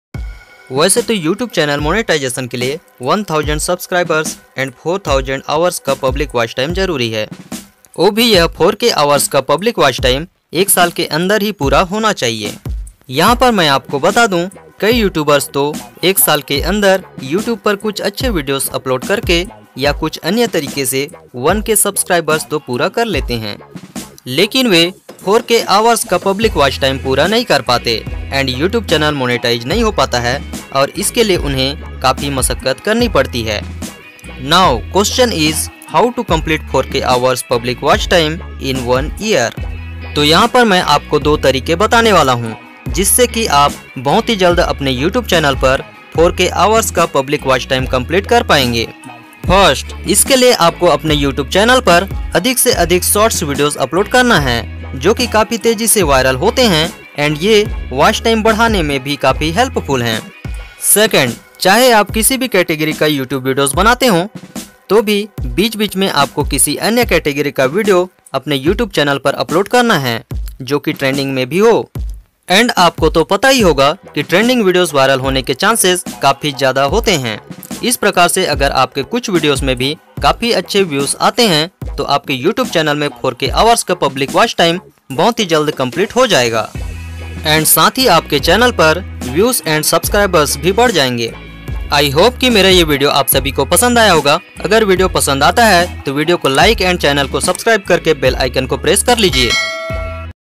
How to Complete 4000 hours Public Watchtime on YouTube | Monetize YouTube Channel
How to Complete 4k hours public Watchtime on YouTube and Monetize your YouTube channel in 2021, it's very easy when applying two methods.
Complete 4000 hours Watchtime and 1000 Subscribers in few days | 2 month